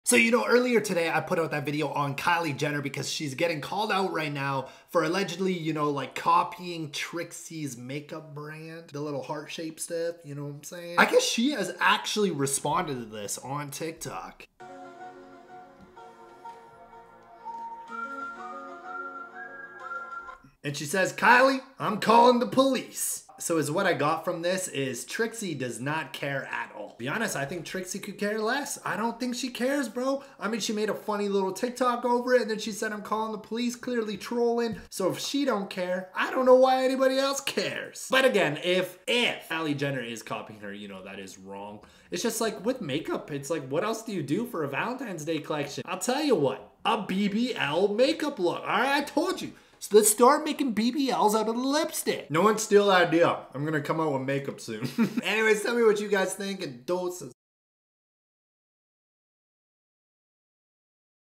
Kylie Jenner Messed Up!! (BUSTED!!)
is Kylie Jenner copying Trixie Mattel cosmetics with her new valentines day line? | Trixie responds on tiktok after many are accusing Kylie Jenner of stealing the heart shaped stuff | KUWTK